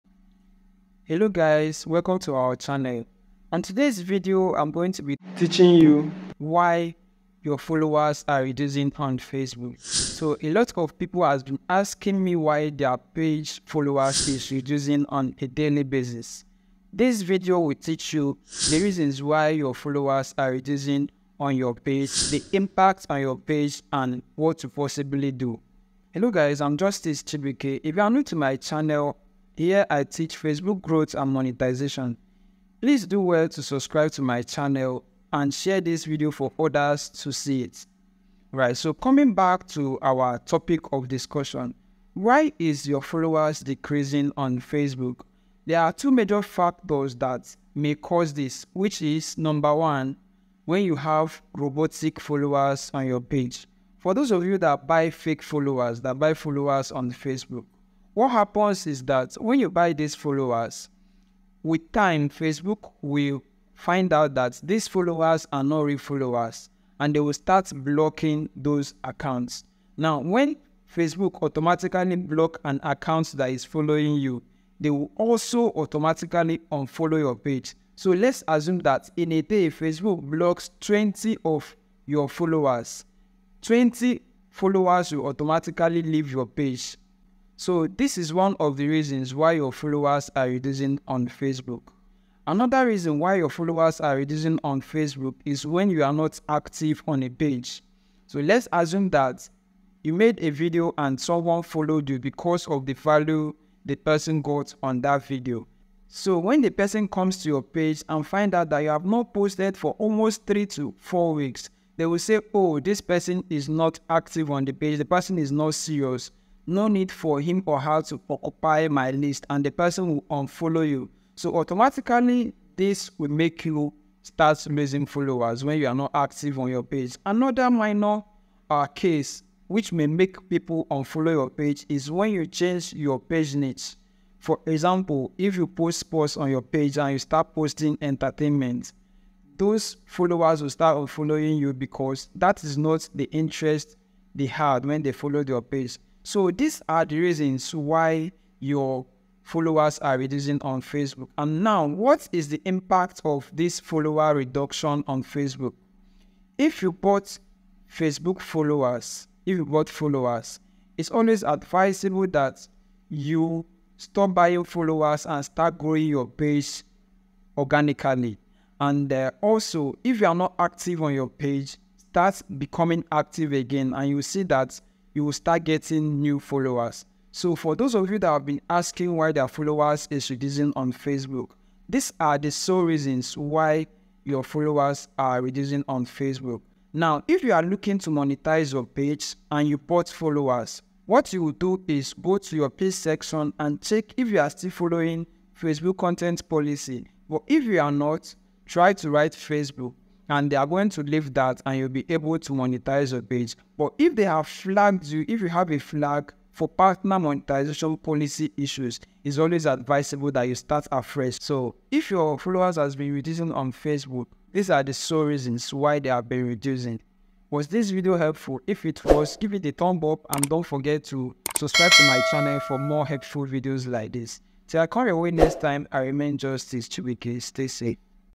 Reasons Why Your Facebook Followers is Reducing and How to Solve it
This video answer the question "why is my Facebook followers Reducing"